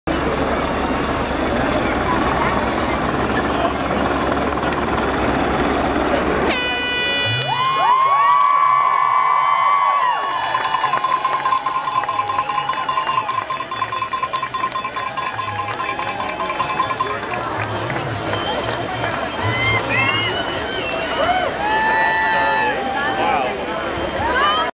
2011 Canada Subaru Ironman Mass Swim Start Part 1
Amazing start of the Canadian Ironman Swim on August 28th, 2011 from Penticton, BC with over 2800 athletes from all over the world participating. The pros started at 0645 with the remaining athletes following at 0700. Shot in HD.